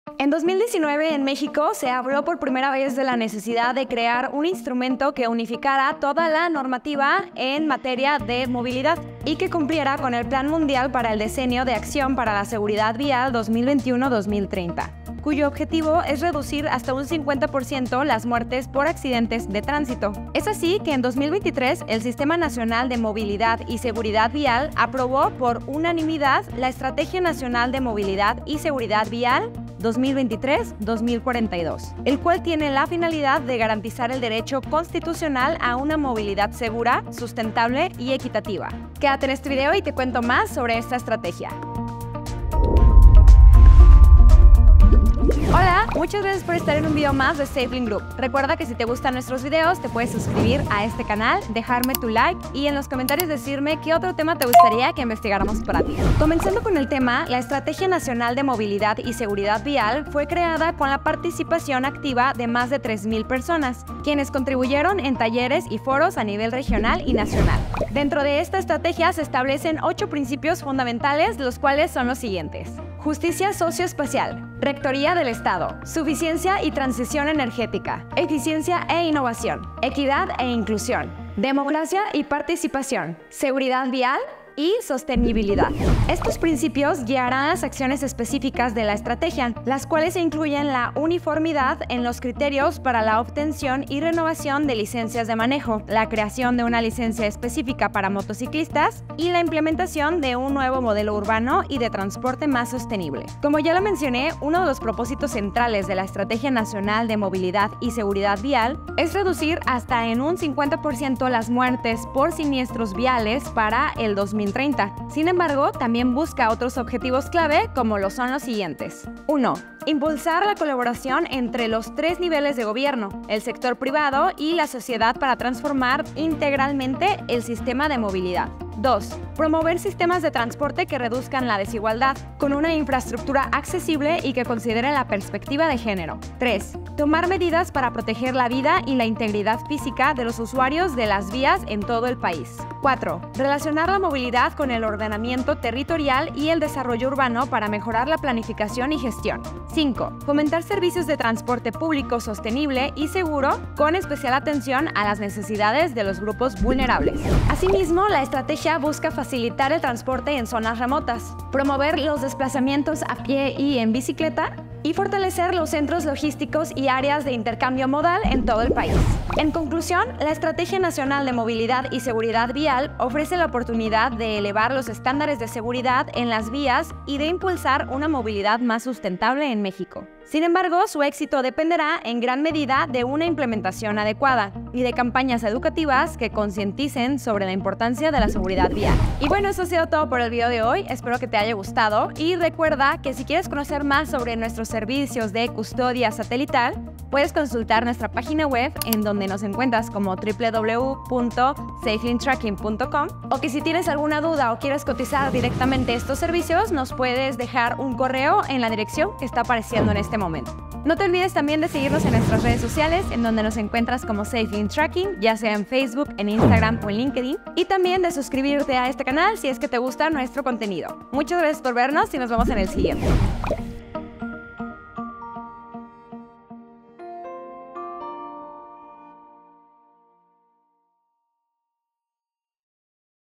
Estrategia Nacional de Movilidad y Seguridad Vial
En una sociedad cada vez más conectada y dinámica, una circulación eficiente y accesible es fundamental para garantizar la accesibilidad a servicios básicos, la integración social y la competitividad de las ciudades y regiones.

Es por ello que recientemente se publicó la Estrategia Nacional de Movilidad y Seguridad Vial 2023–2042, la cual sienta las bases para un mejor desarrollo de estos ámbitos en México por los próximos 19 años.